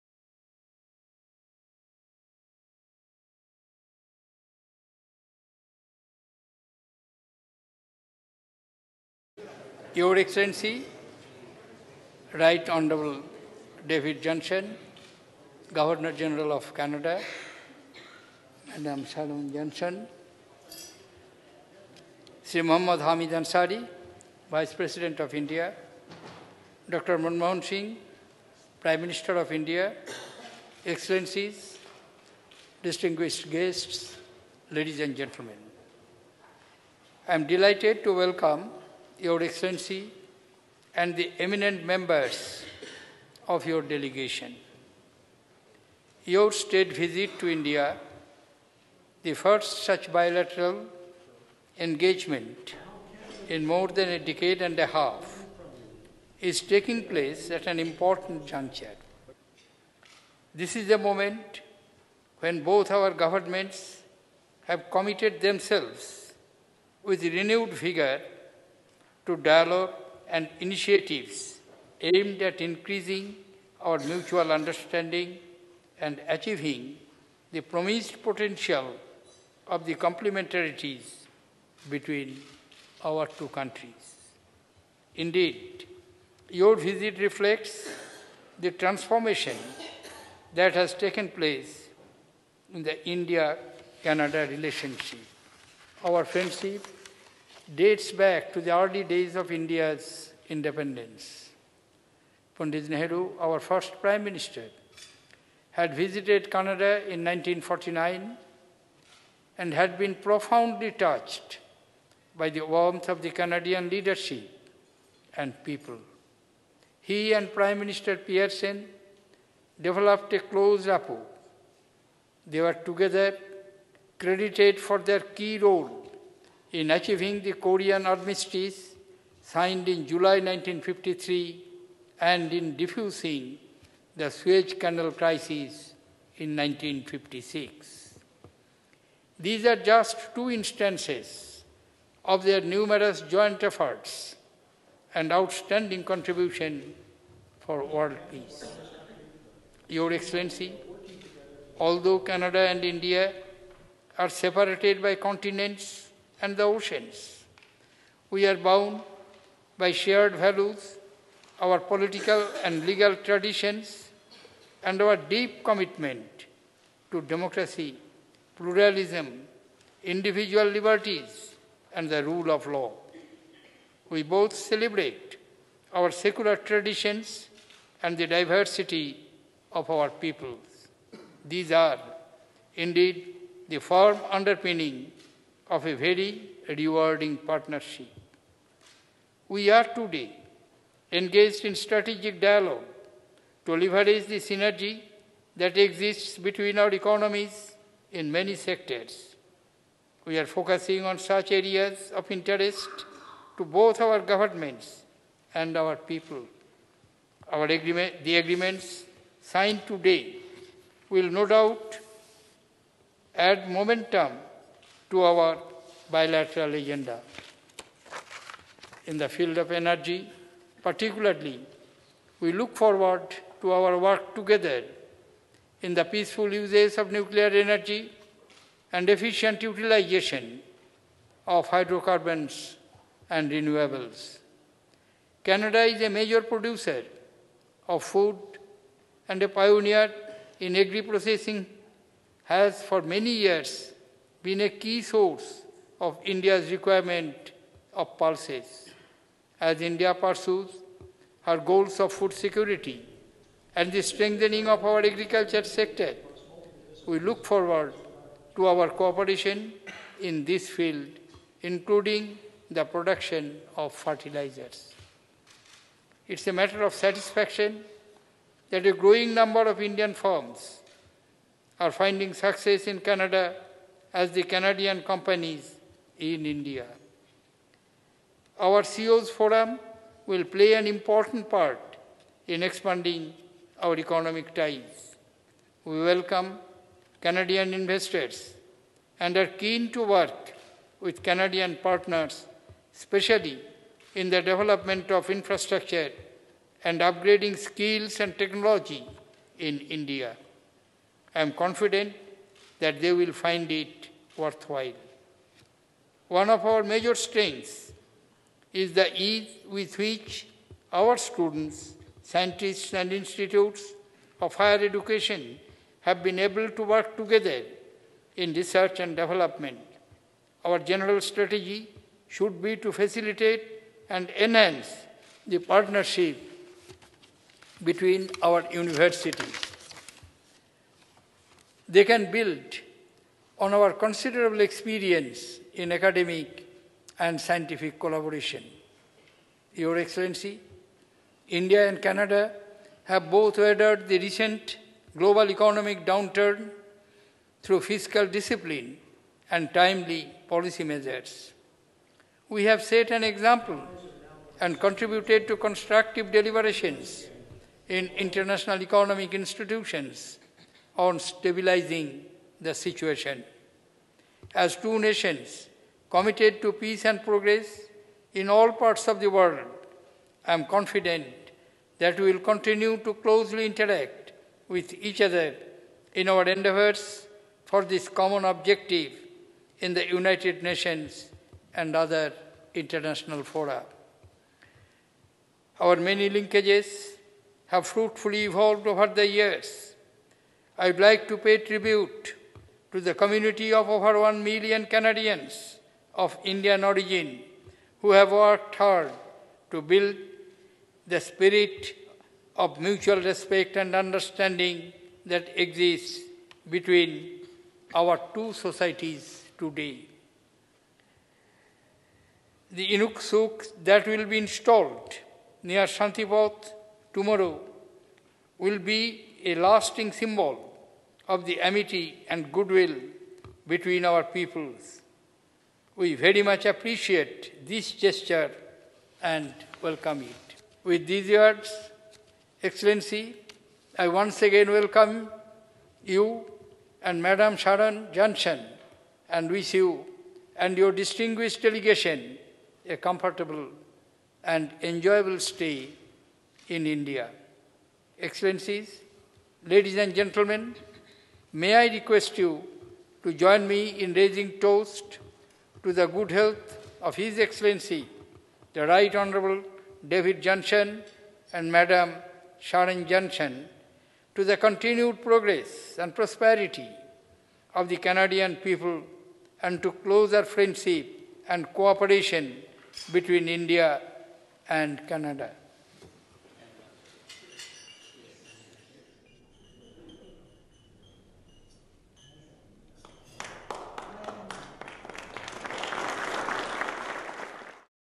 President Mukherjee host Banquet in honour of The Rt. Hon David Johnston, Governor General of Canada
H.E. The Rt. Hon. David Johnston, Governor General of Canada and Mrs. Sharon Johnston called on the President of India, Shri Pranab Mukherjee yesterday (February 24, 2014) at Rashtrapati Bhavan. The President also hosted a banquet in their honour following the meeting.

Speaking on the occasion, the President said Canada has been a partner in the initial stages of India’s development. Pandit Nehru had a personal friendship with Prime Minister of Canada Lester Pearson. Massanjore dam, across the Mayurakshi River in Dumka district of Jharkhand, commissioned in 1956, was built with Canadian aid. It is popularly known as Canada Dam or Pearson Dam and is one of the best examples of Canadian contribution to India’s development. The President said India is interested in receiving investments and technological know-how from Canada, especially in the field of infrastructure, mining, etc. where Canada has well-developed technical expertise. Indian public sector companies are also interested in opportunities in oil and natural gas in Canada, as well as in purchasing stakes in energy assets.

In his banquet speech, the President said the State Visit of the Governor General of Canada, the first such bilateral engagement in more than a decade and a half, is taking place at an important juncture. This is a moment when both Governments have committed themselves with renewed vigour to dialogue and initiatives aimed at increasing mutual understanding and achieving the promising potential of complementarities. He said although Canada and India are separated by continents and the oceans, the two countries are bound by shared values, political and legal traditions and deep commitment to democracy, pluralism, individual liberties and the rule of law. Both the countries celebrate secular traditions and the diversity of their peoples. These are the firm underpinning of a very rewarding partnership.

The President said in the field of energy India looks forward to work together with Canada in the peaceful uses of nuclear energy and efficient utilization of hydrocarbons and renewables. As India pursues its goals of food security and the strengthening of agriculture sector, it looks forward to co-operation in this field, including in the production of fertilizers.

The President said many linkages of India and Canada have fruitfully evolved over the years. He paid tribute to the community of over one million Canadians of Indian origin who have worked hard to build the spirit of mutual respect and understanding that exists between the two societies. He said the "Inukshuk” that will be installed near Shanti Path in New Delhi will be a lasting symbol of the amity and goodwill between peoples of India and Canada.

Speaking at the welcome banquet, the Governor General of Canada expressed the hope that his visit would further strengthen the bonds between the two nations, steeped in traditions of parliamentary democracy, pluralism and strong people to people ties. He said Canadians of Indian origin, numbering more than a million, are contributing greatly to Canada’s success.